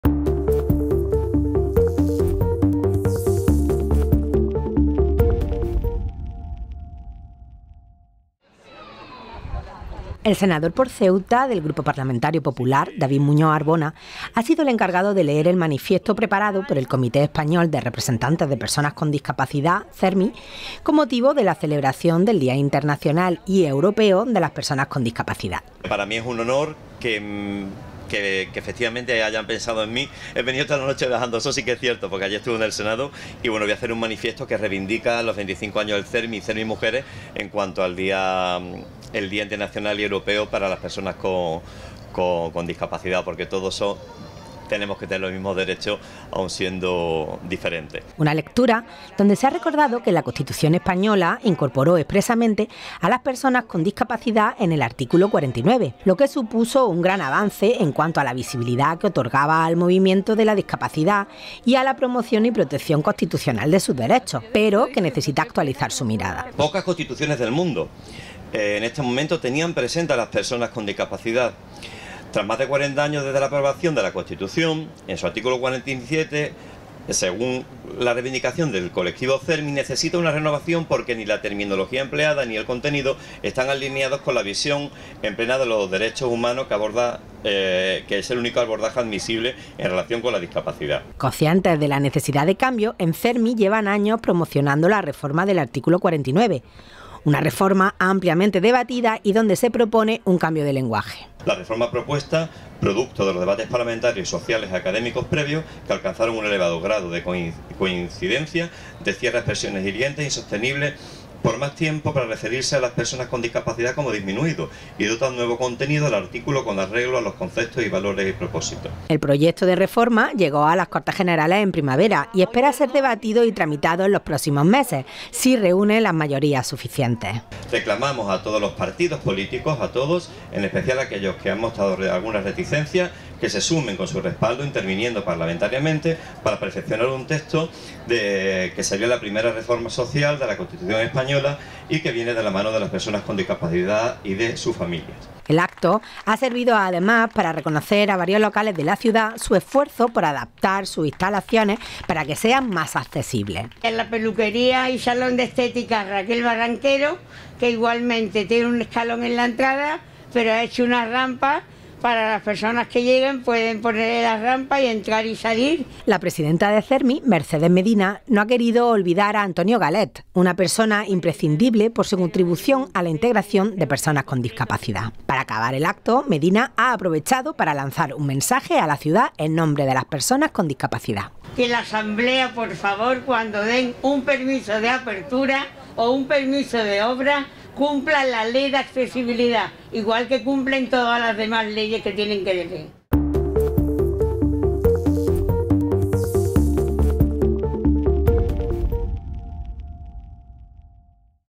El CERMI solicita que se cumplan las leyes de accesibilidad en los locales de Ceuta
El Comité ha celebrado un acto con motivo del Día Internacional y Europeo de las Personas con Discapacidad con la lectura de un manifiesto por parte del senador por Ceuta, David Muñoz Arbona, en el que piden la renovación del artículo 49 de la Constitución

Redacción y locución: Mayte Solán
Imagen y edición: Mauro Mancebo